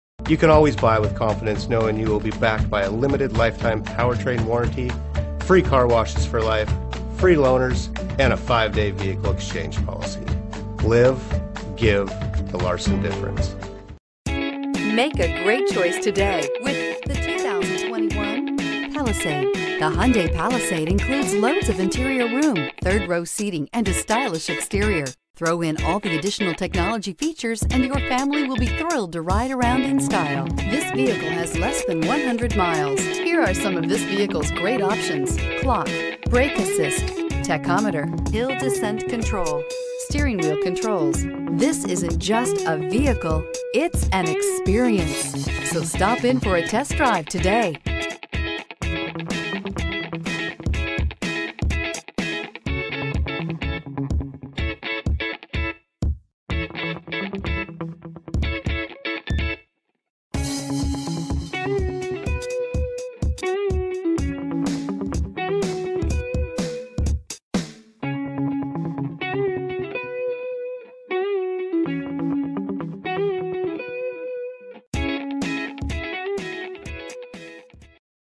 2021 Hyundai Palisade H3587
2021 Hyundai Palisade SEL

Larson Hyundai
7601 S. Tacoma Way

With only 9 miles, this model provides an impressive 19/24 MPG city/highway. If youre looking for power, this Hyundai Palisade SEL has it. You can find a Regular Unleaded V-6 3.8 L/231 engine paired with a 8-Speed Automatic w/OD transmission. Technology options includes: 1 LCD Monitor In The Front, Radio w/Seek-Scan, Clock, Speed Compensated Volume Control, Steering Wheel Controls, Voice Activation and Radio Data System.